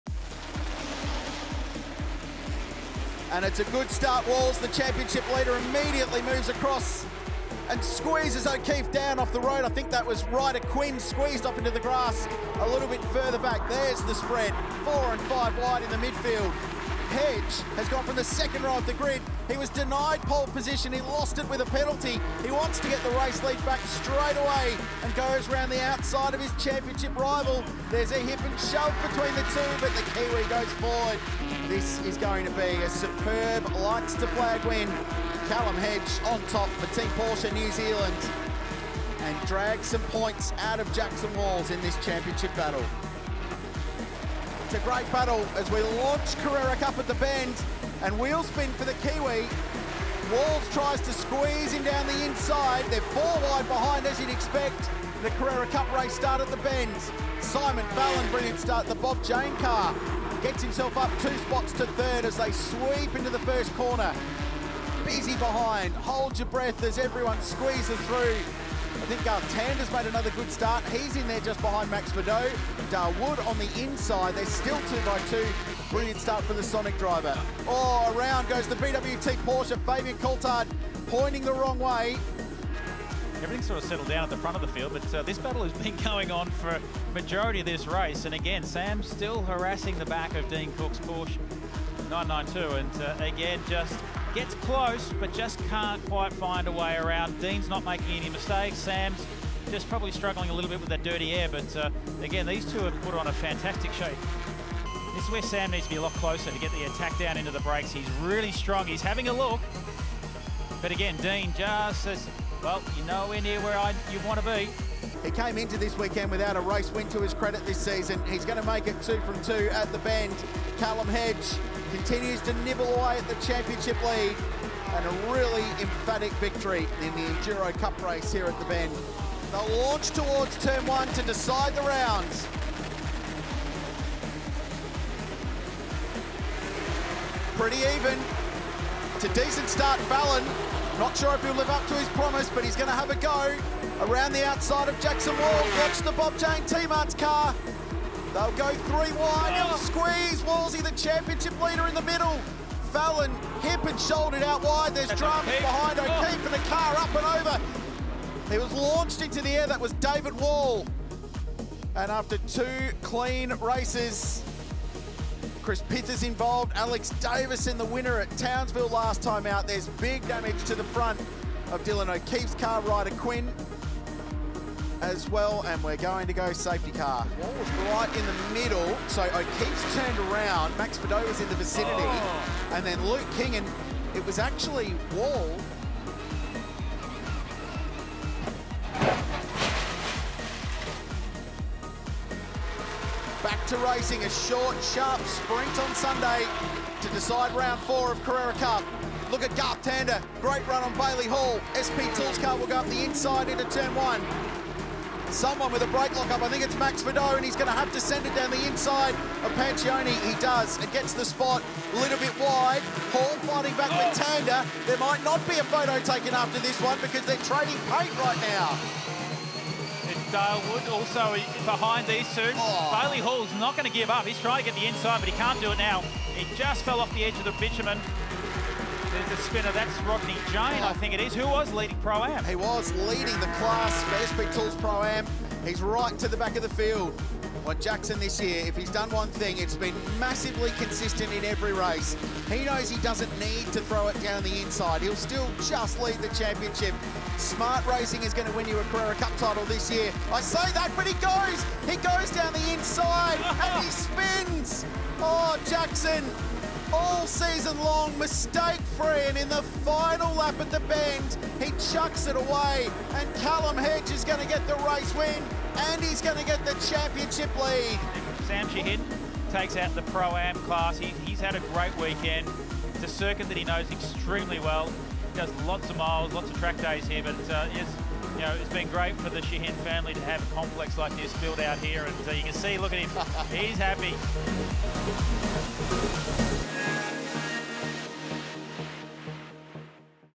EXTENDED HIGHLIGHTS: The Bend Motorsport Park, 2023
Intense starts, close racing and THAT final lap in Race 3 - relive all the highlights from another wild Porsche Paynter Dixon Carrera Cup Australia round at The Bend.